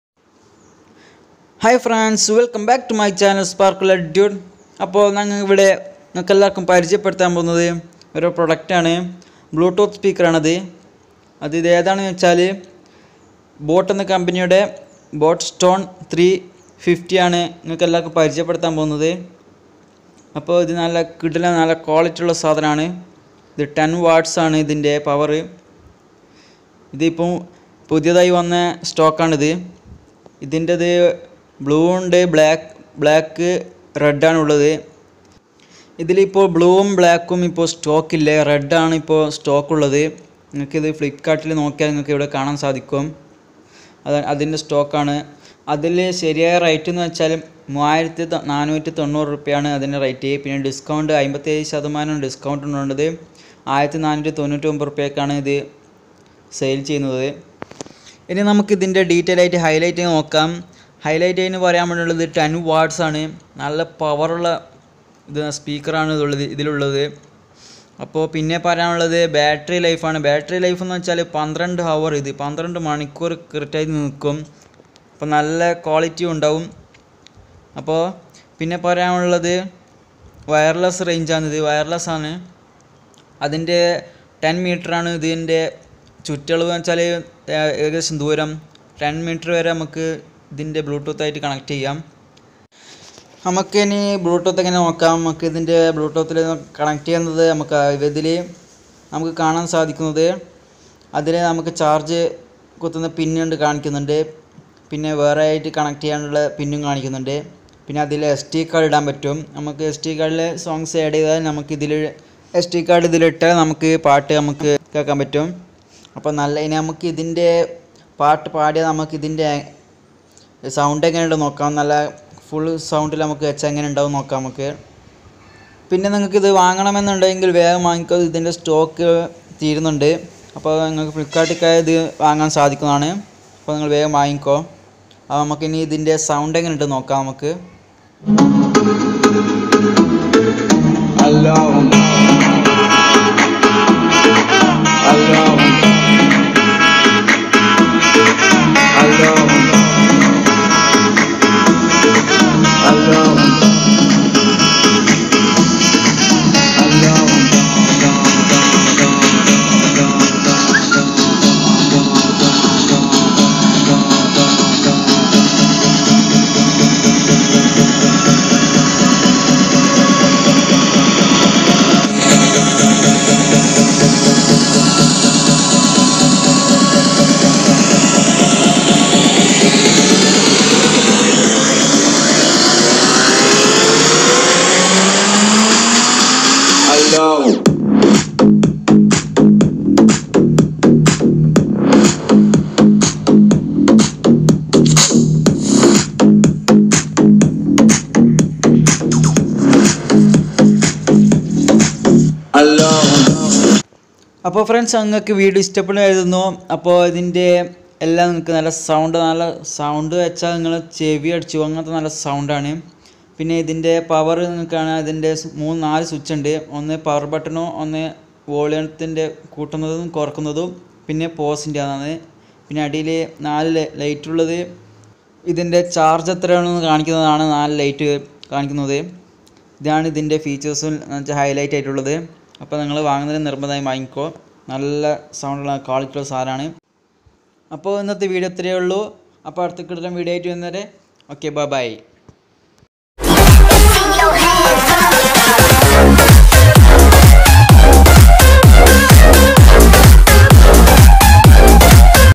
BoAt Stone 350 || 10W Bluetooth Speaker Unboxing and Review in Malayalam || sound test || ₹1,499
Send your feedbacks.